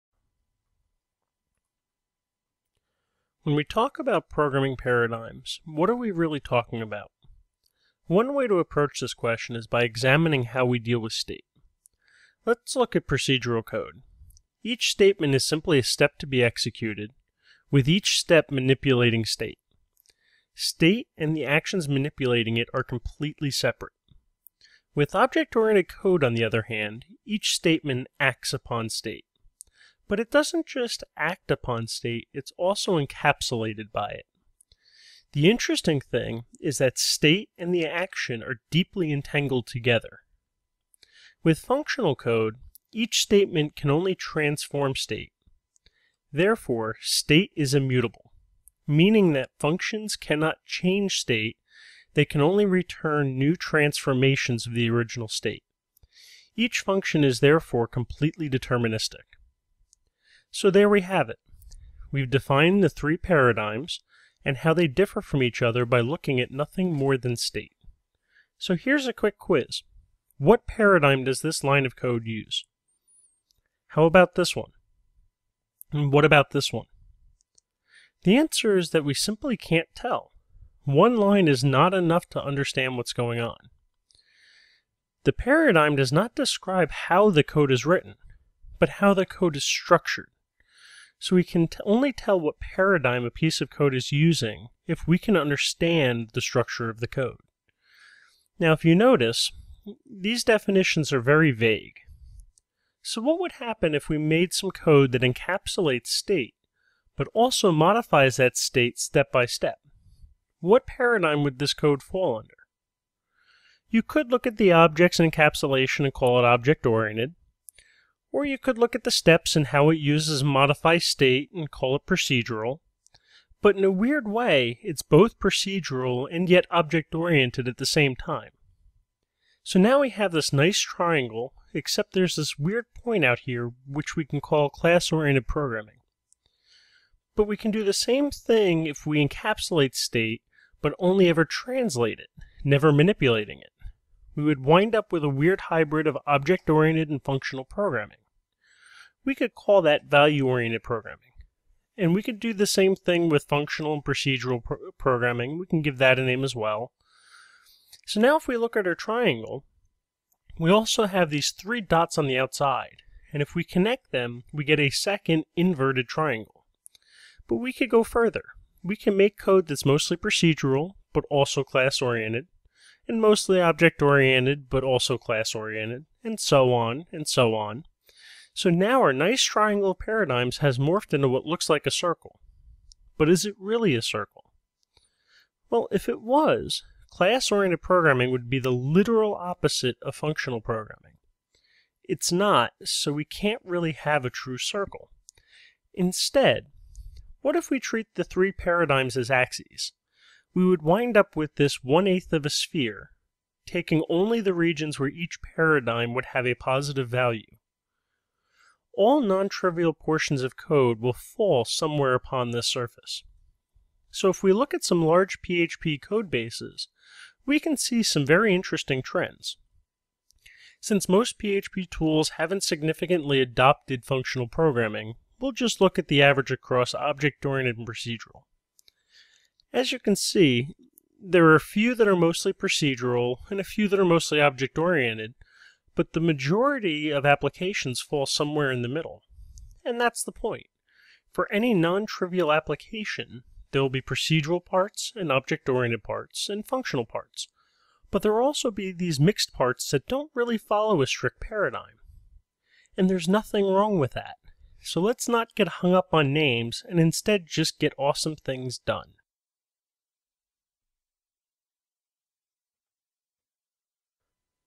Programming With Anthony - Paradigm Soup
Programming paradigms are fun to debate and talk about. In this short video, we'll talk about the differences between Procedural, Object Oriented and Functional programming. We'll also explore what happens when these paradigms cross over each other.

This is the first of what I hope to be many videos on the topic of programming. Plenty of future ones are planned, but the topics, order and even if they happen is completely up to you! I want your feedback on the concept. Is it something worth pursuing? Any topics you'd like to see? Any other comments about the concept?